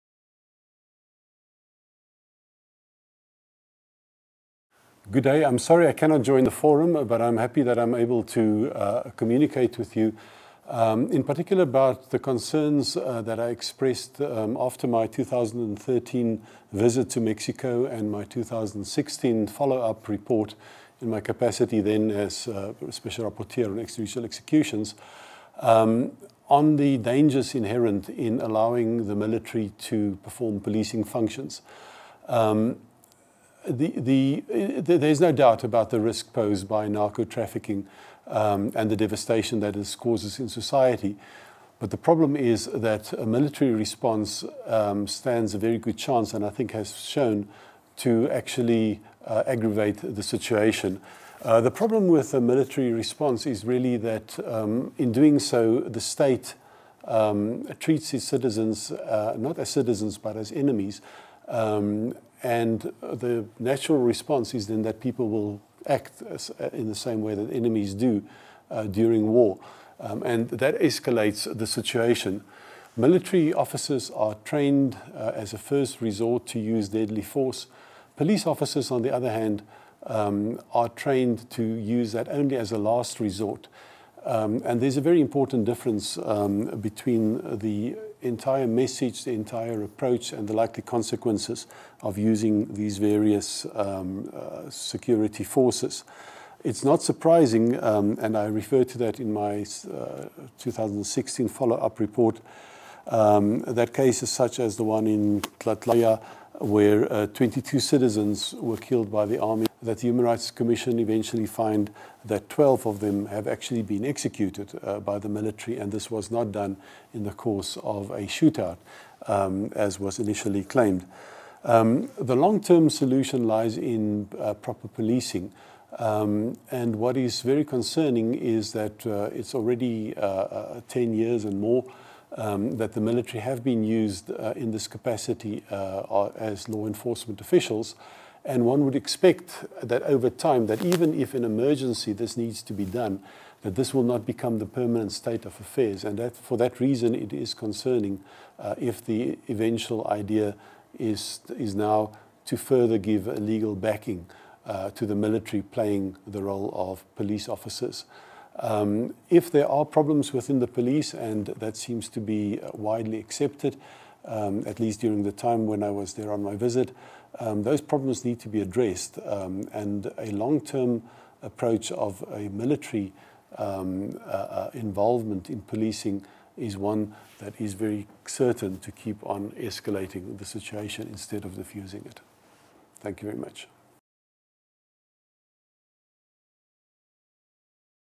Participación de Christof Heyns en la Mesa sobre Seguridad y DDHH en México
Durante la Mesa sobre Seguridad y DDHH en México, el ex-relator de Ejecuciones Extrajudiciales, participó a través de un pequeño video.